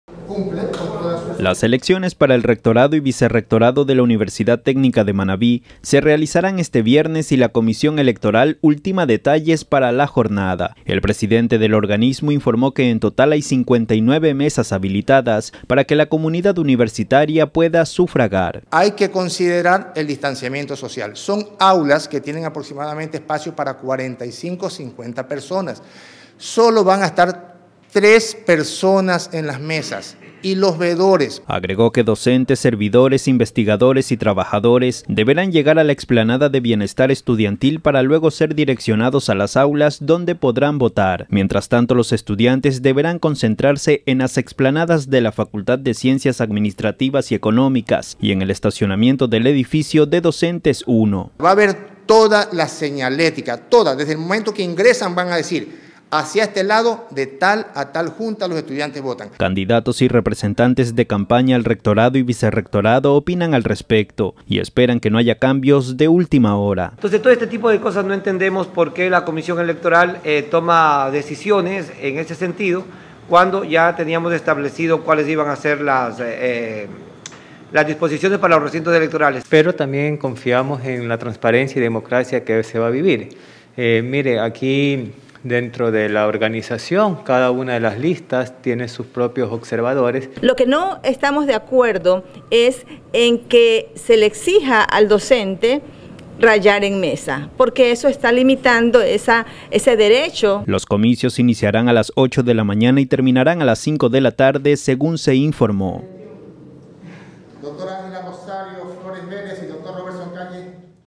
El día de las elecciones habrá 59 mesas habilitadas para ejercer el derecho al voto
Portoviejo